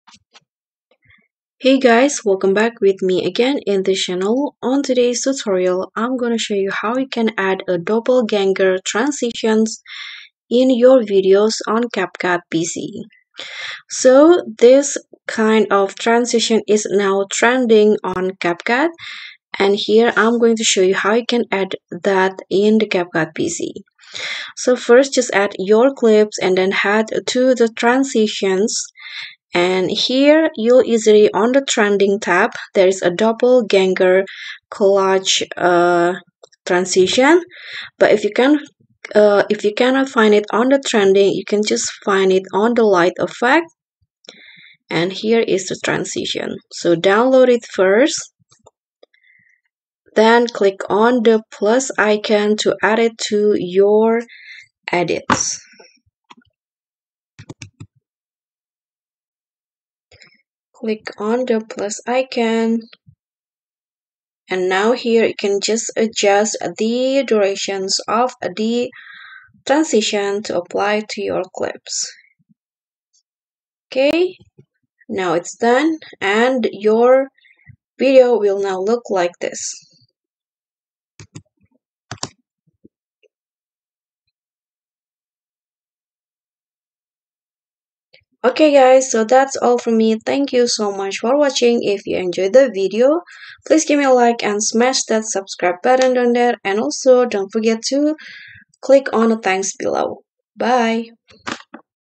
How Can You Easily Add the Doppelganger Collage Transition to Your Edited Videos in CapCut PC?
Try applying this new and trending "Doppelganger Collage" transitions for your next edits in CapCut PC